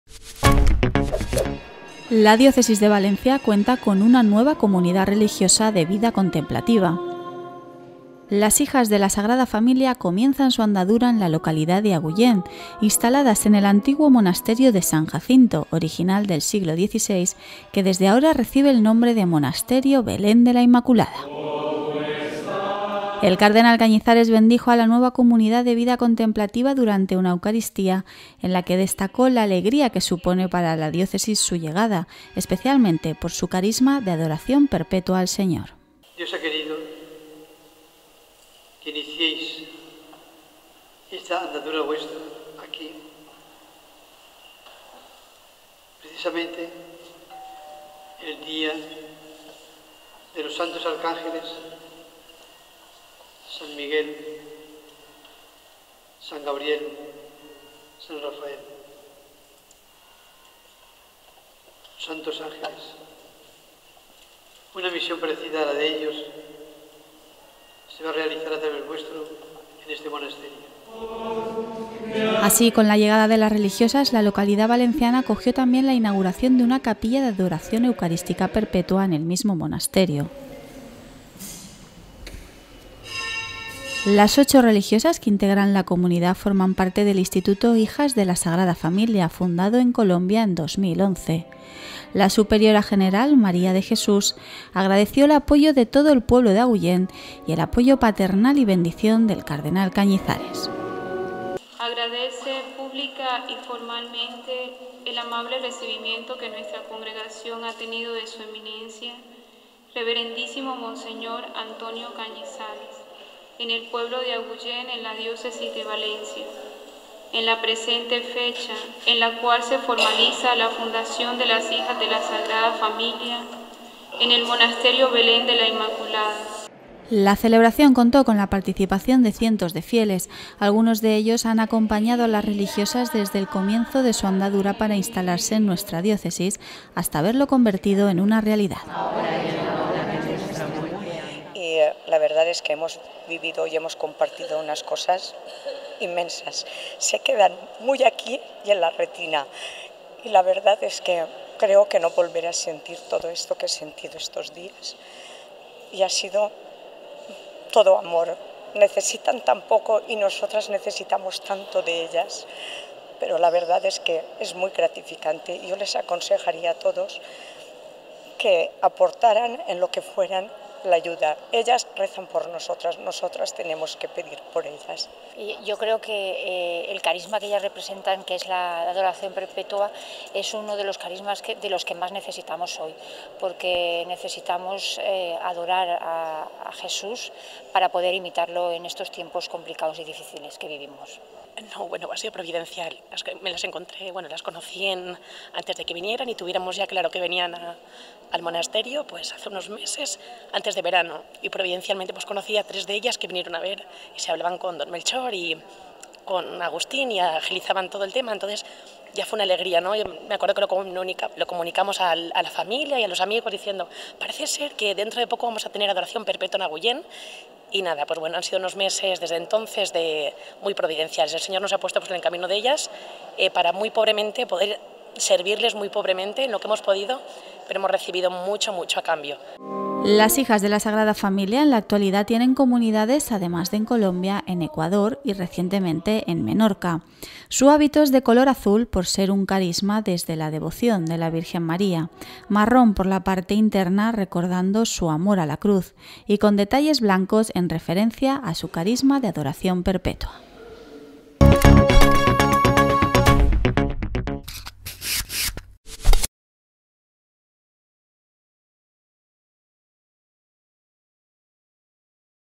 Las Hijas de la Sagrada Familia llegan a Agullent
La diócesis de Valencia cuenta con una nueva comunidad religiosa de vida contemplativa. Las Hijas de la Sagrada Familiar comienzan su andadura en la localidad de Agullent. Con la llegada de las religiosas, la localidad acogió la inauguración de una capilla de Adoración Eucarística Perpetua en el mismo monasterio.